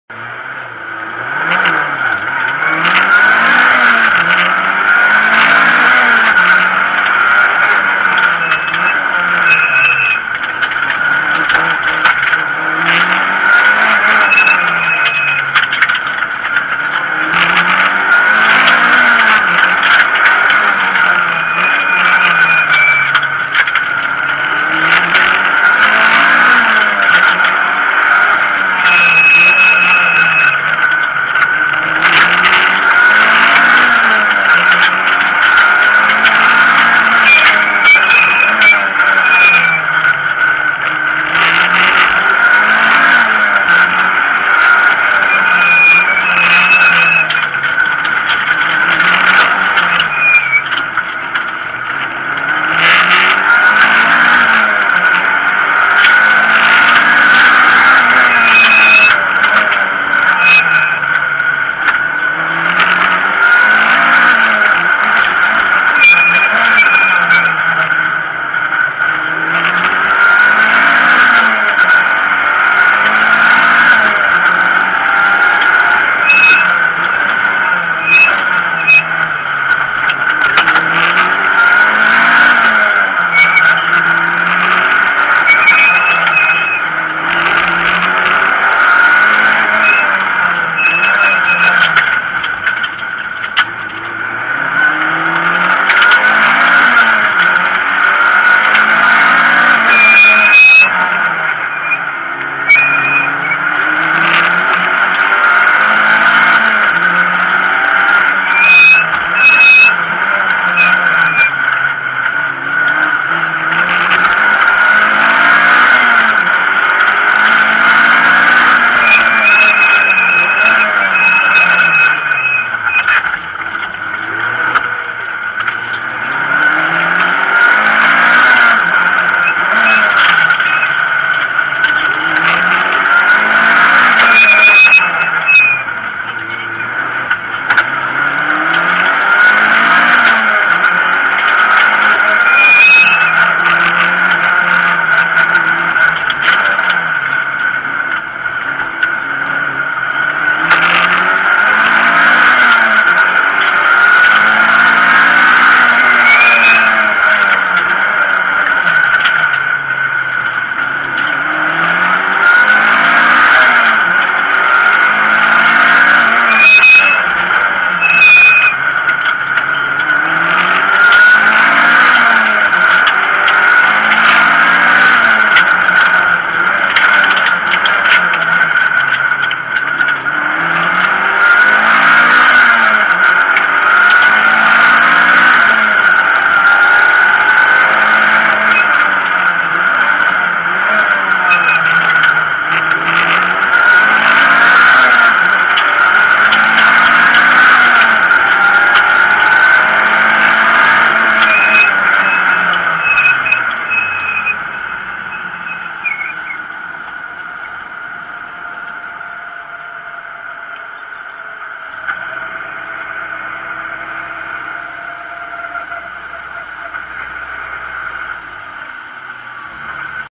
Targa Tas 2011 prologue, Evans/Feaver Evo X
Targa Tasmania 2011 Prologue, in the Hankook TMR Evo X RS of Dean Evans and Toni Feaver. Time of 3m:20.8, being 12th fastest. Car runs a five-speed manual, E85 fuel and weighs approx 1470kg without driver, on Hankook Ventus TSZ221 235/45R18 R spec tyres.